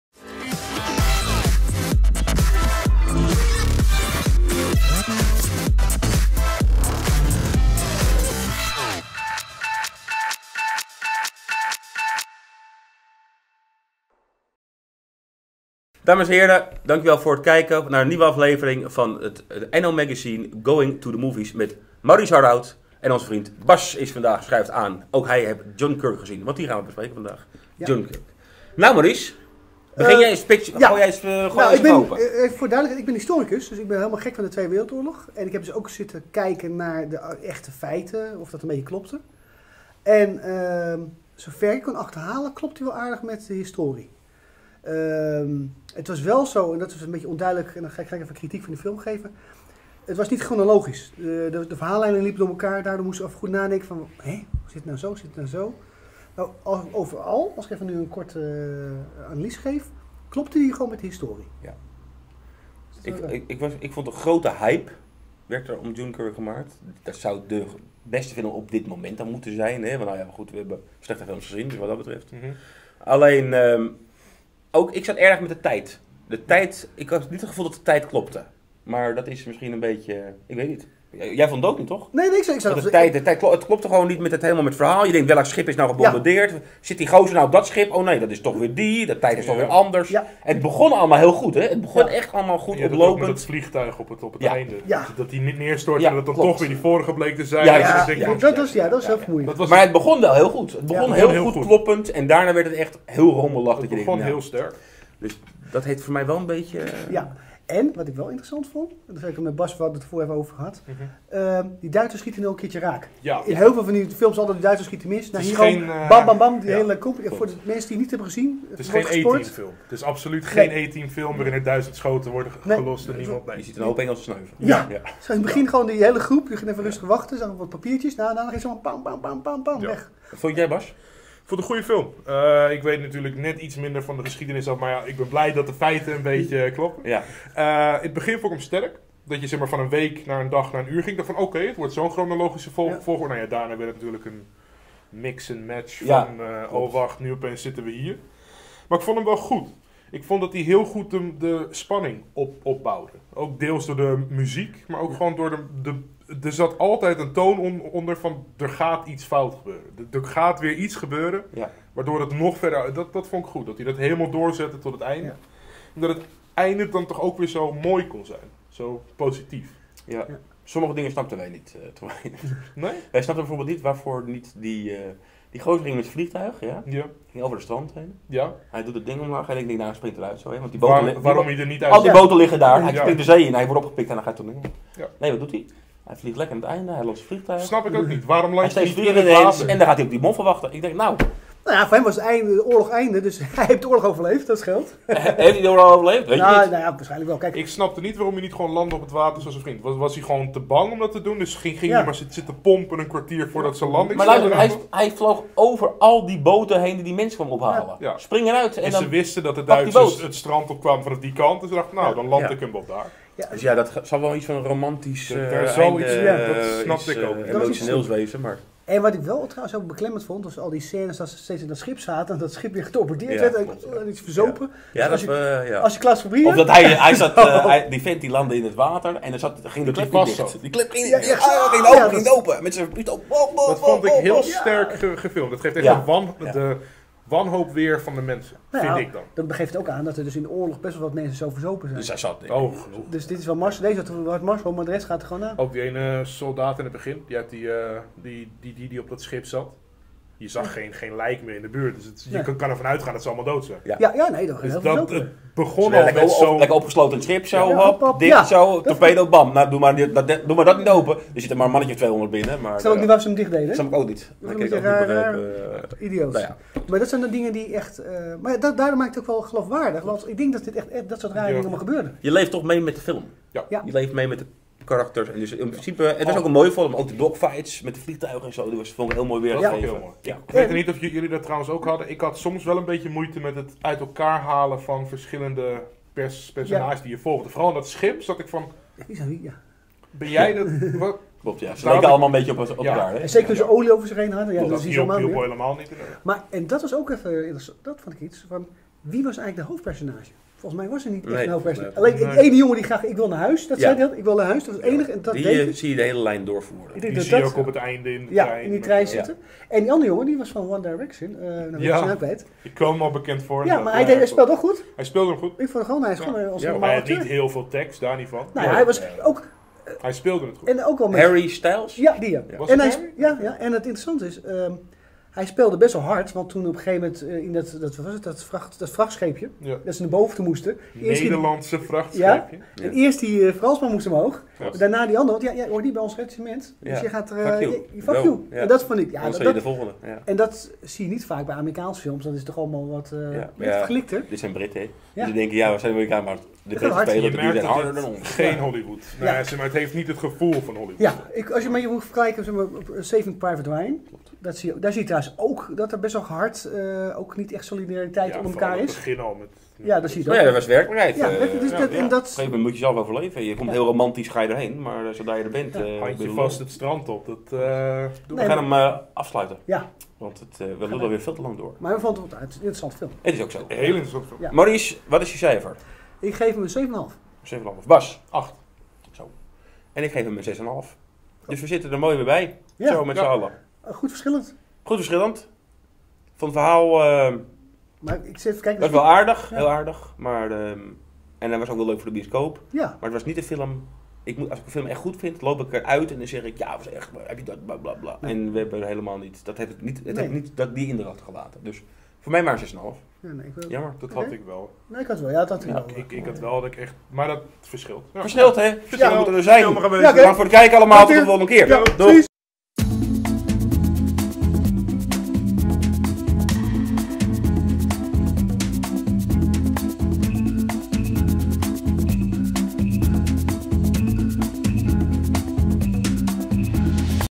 Review Dunkirk Nederlands
Patrick van de Wijk bespreekt samen met Maurice Harroud en Bas Out de film Dunkirk van Christopher Nolan. De film gaat over de extractie van de Britse en Franse troepen vanuit Duinkerken in 1940. De geallieerden zitten opgesloten en alleen een wonder kan hen helpen. Deze gebeurtenis wordt dan ook als "het wonder van Duinkerken" beschreven in de geschiedenisboeken.